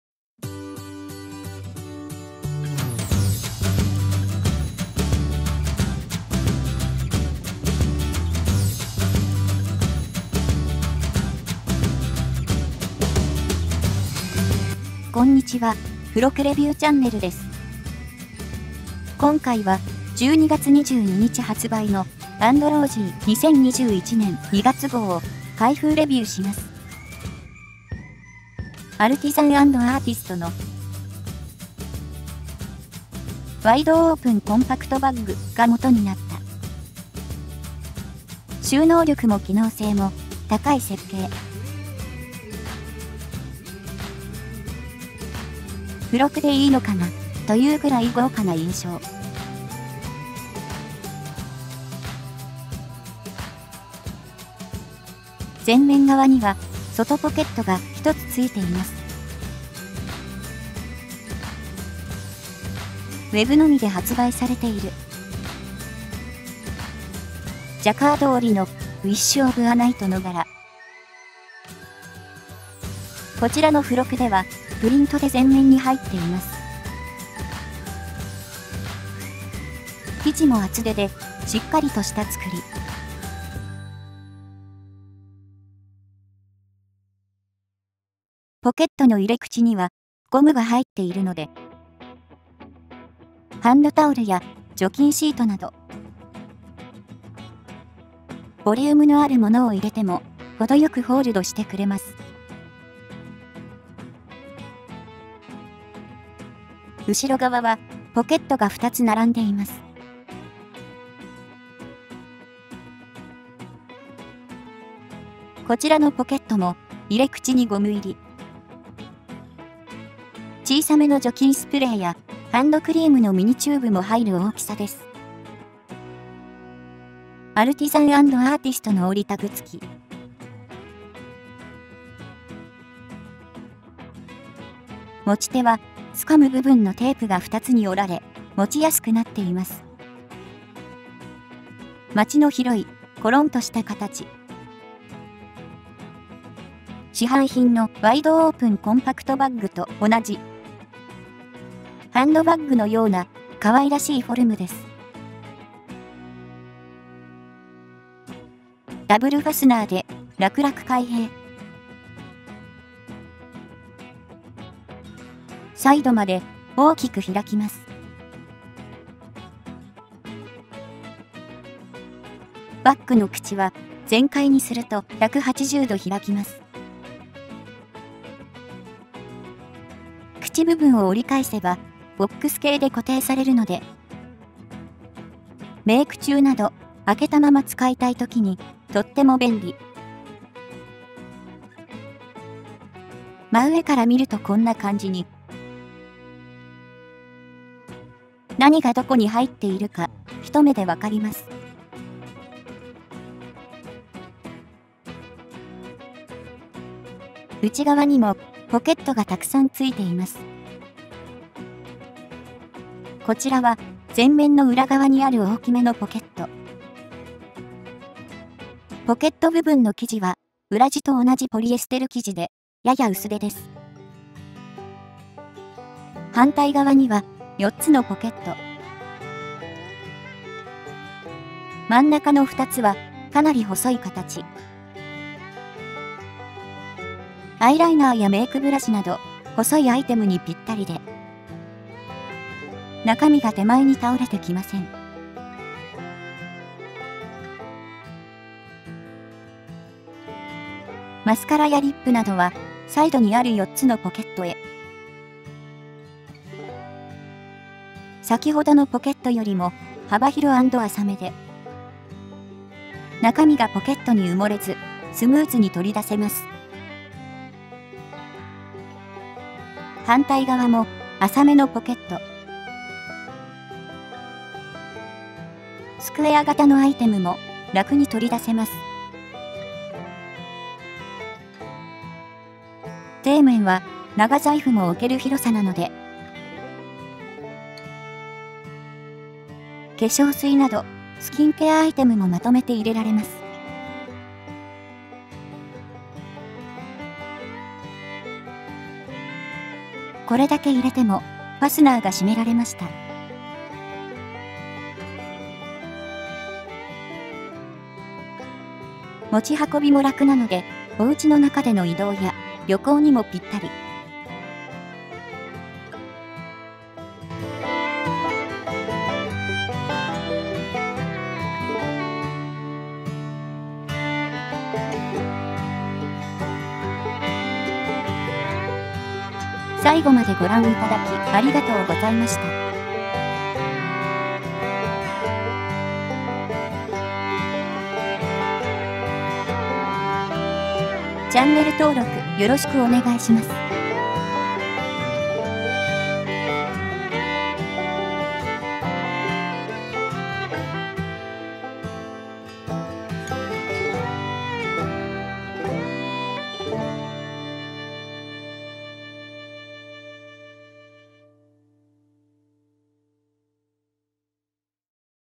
【雑誌付録】& ROSY 2021年2月号 アルティザン・アンド・アーティスト 大容量＆ポケットいっぱい ポーチバッグ
【開封レビュー】アンドロージー
発売日：2020年12月22日
出版社：宝島社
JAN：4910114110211
価格：1,650円（税込）

サイズ(約): H19×W19×D15cm
素材：ポリエステル裏PVC貼り加工、ポリエステル、PP、合金、鉄

アルティザン・アンド・アーティスト　ARTISAN&ARTIST